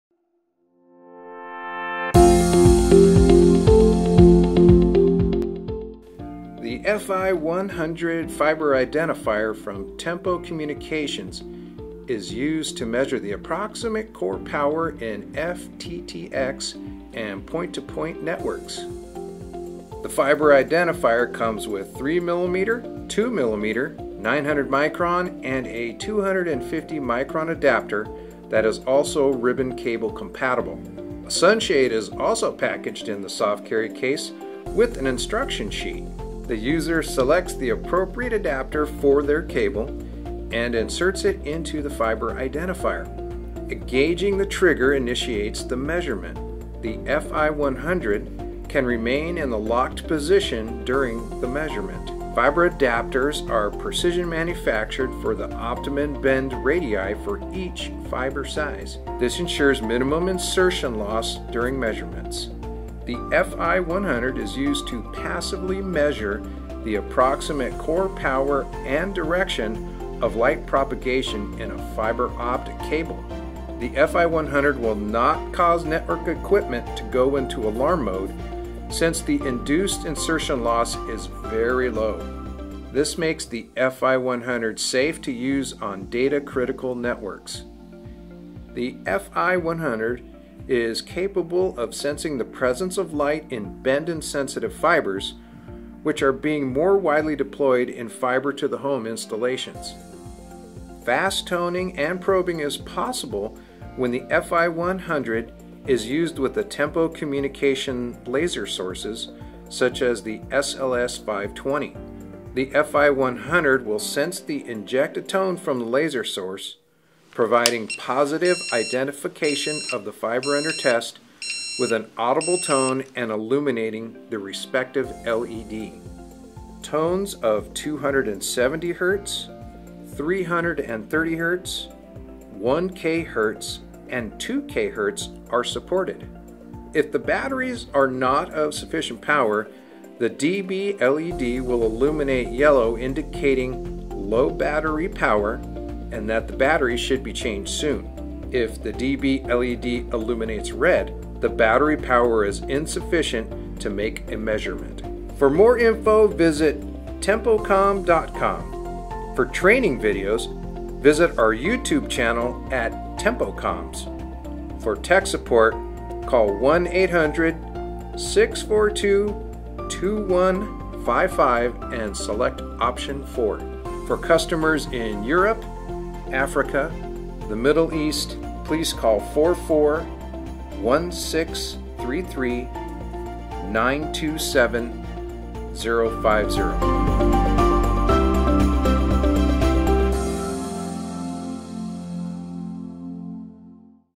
Tempo FI-100 Training Video
The FI-100 fiber identifier allows the technician to quickly determine the approximate core power and signal direction in a fiber optic cable without disconnecting the fiber. This training video goes more in-depth on the FI-100.